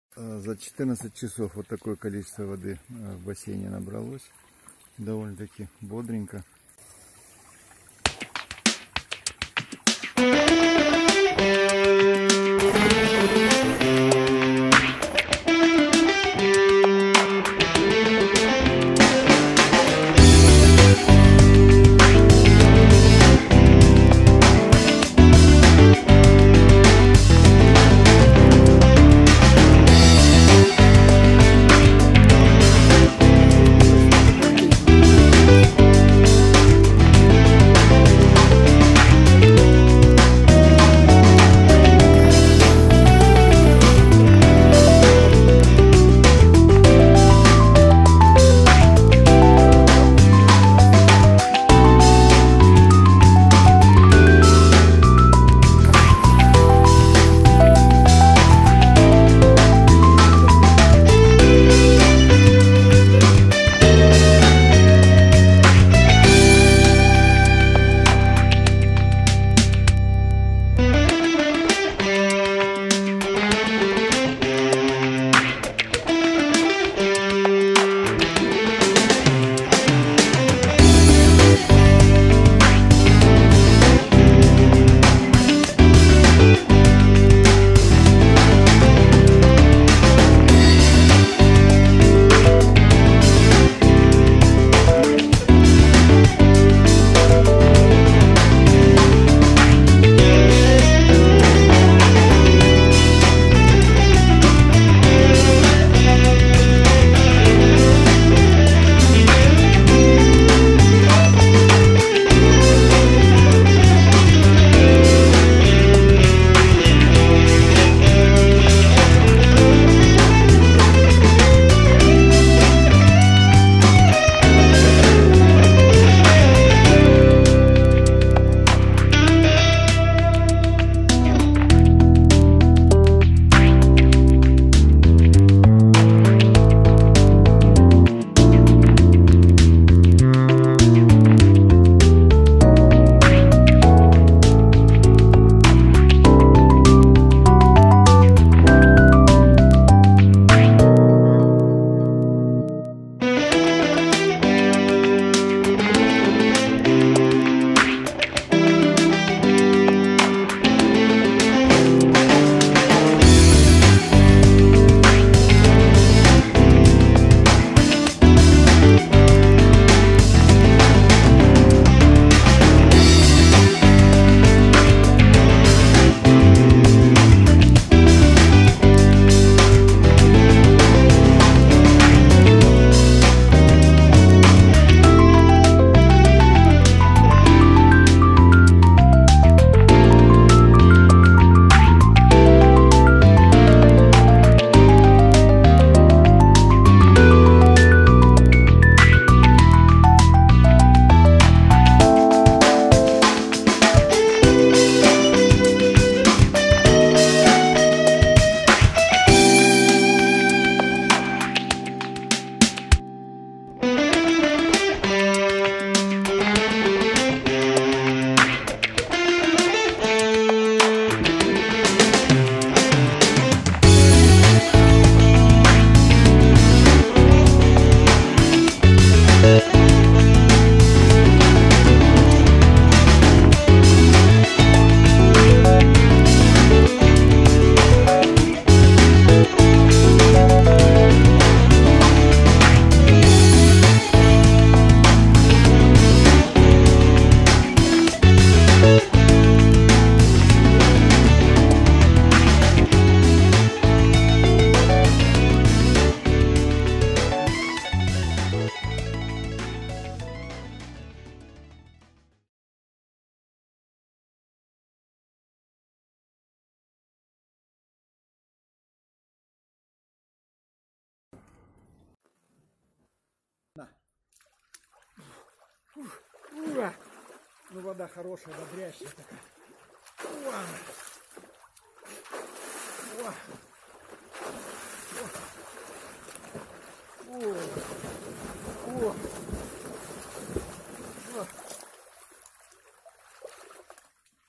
Погружаемся в минувшее лето
В летнюю жару купили каркасный бассейн установили быстренько, и как начали купаться! Прыгали, ныряли, плавали наслаждались летом! Впереди новый купальный сезон, и мы скоро вновь будем устанавливать каркасный бассейн чтобы прятаться в нем от жары! это гораздо дешевле, чем поездка на море или